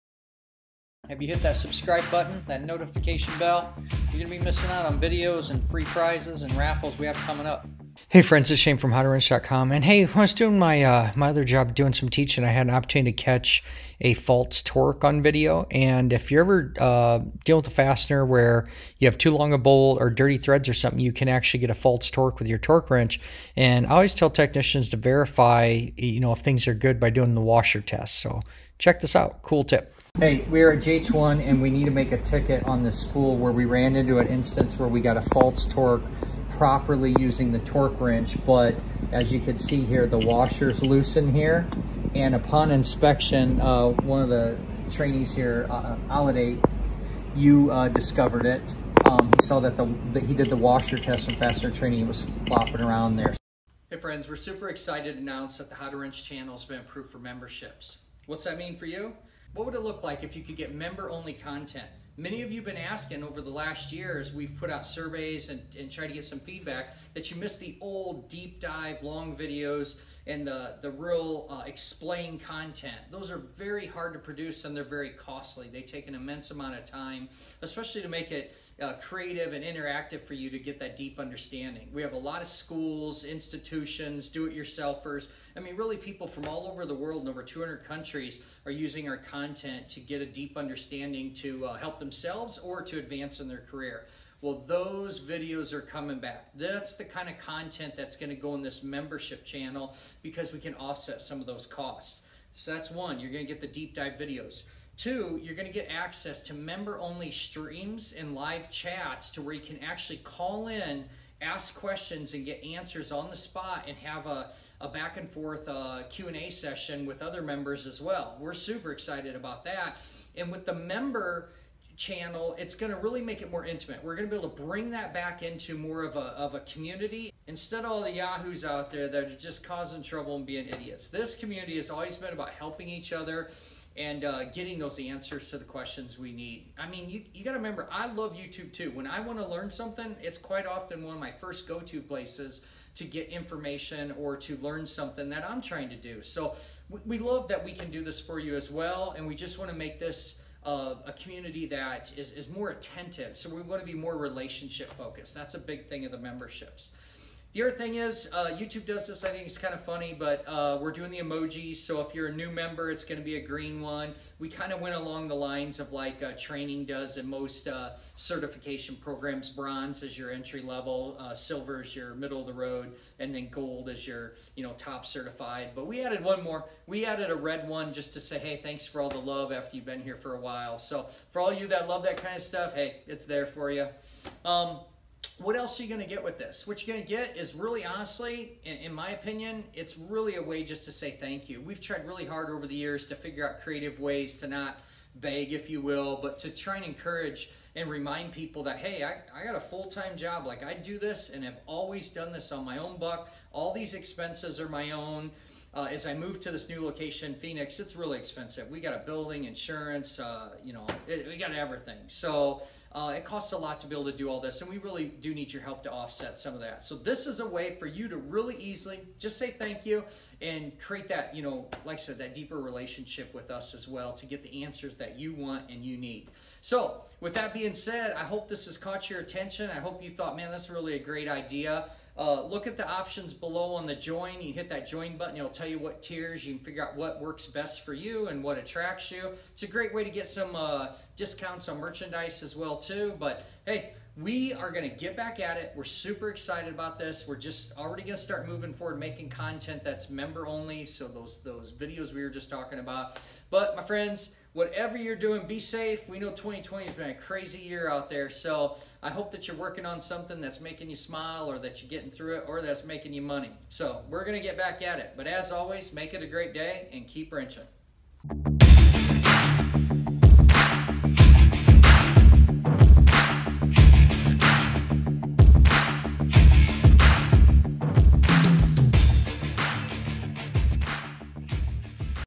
Why do my bolts not stay tight? How to identify a false torque with the "washer test"
Here is a simple test you can do in seconds to make sure you're not getting a false torque after you tighten any fastener with a washer used on either side of the assembly.
Keep Wrenching,
Shane Conley
Instructor/Motivator/Motorcyclist